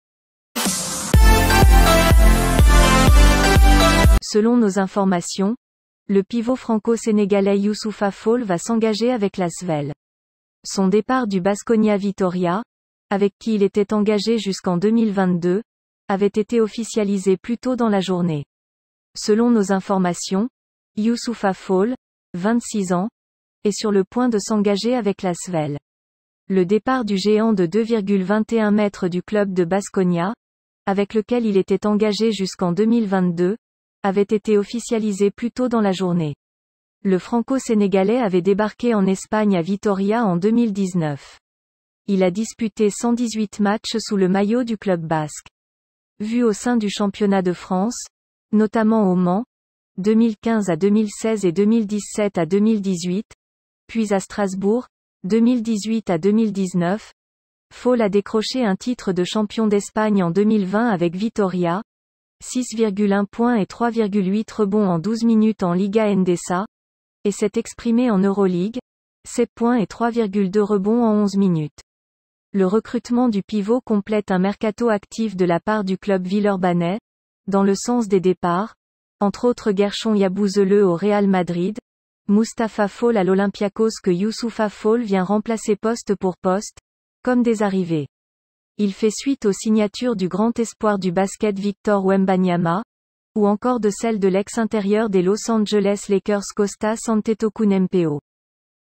Youssoupha Fall va s'engager avec l'Asvel
Selon nos informations, le pivot franco-sénégalais Youssoupha Fall va s'engager avec l'Asvel. Son départ du Baskonia Vitoria, avec qui il était engagé jusqu'en 2022, avait été officialisé plus tôt dans la journée.
Selon nos informations, Youssoupha Fall (26 ans) est sur le point de s'engager avec l'Asvel. Le départ du géant de 2,21 m du club de Baskonia, avec lequel il était engagé jusqu'en 2022, avait été officialisé plus tôt dans la journée. Le Franco-Sénégalais avait débarqué en Espagne à Vitoria en 2019. Il a disputé 118 matches sous le maillot du club basque.